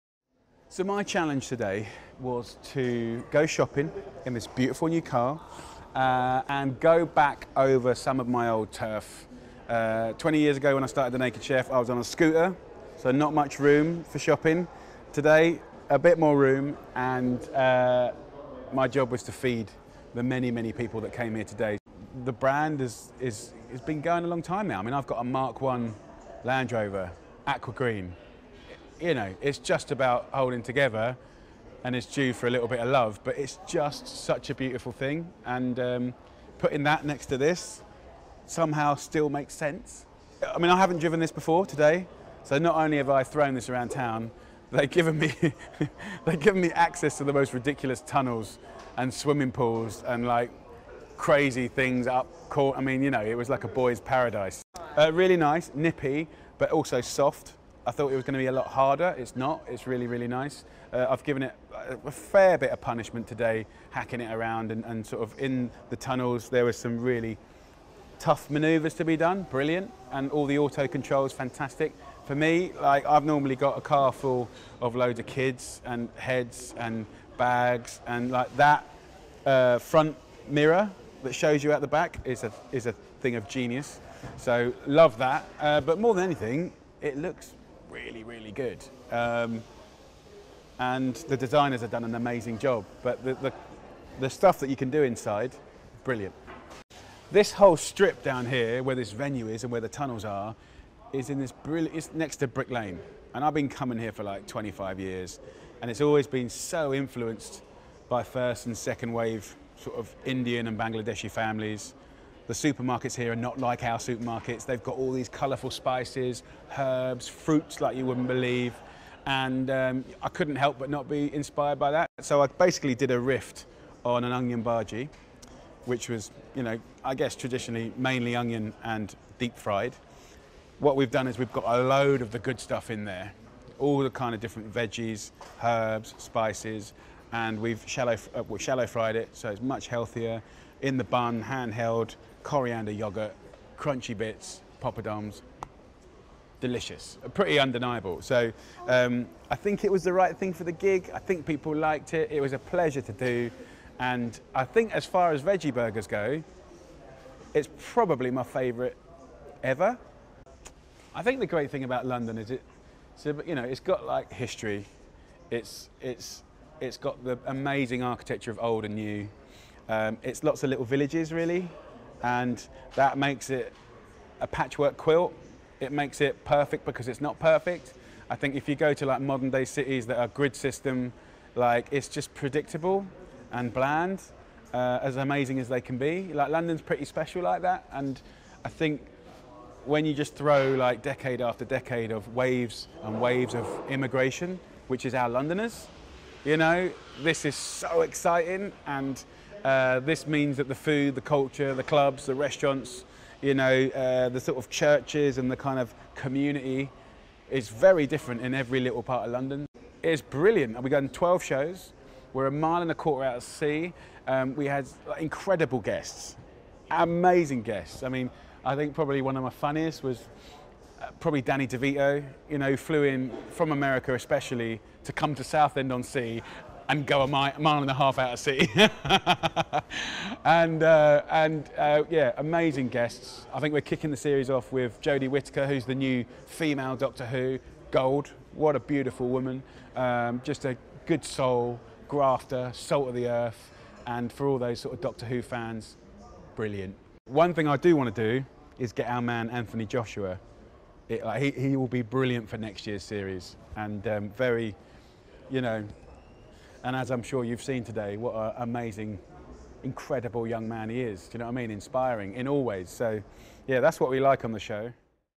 Jamie Oliver The New Range Rover Evoque Launch en
Celebrity chef Jamie Oliver became one of the first people to drive the new Range Rover Evoque, visiting some of his favourite London foodie spots for ingredients to create the city’s best veggie burger.

As part of the global reveal of the new Range Rover Evoque, Jamie navigated London’s busy streets to pick up everything he needed to make his Indian-inspired bhaji burger, which he then cooked for guests at Land Rover’s Evoque Experience.

In a throwback to his Naked Chef days, Jamie visited Brick Lane, stopping at Taj Stores for spices and yoghurt and Beigel Bake to collect bread rolls. Taking a shortcut through Bishopsgate Goods Yard, he put the Range Rover Evoque through its paces, wading through water pools and taking the car across a series of ramps through the tunnels.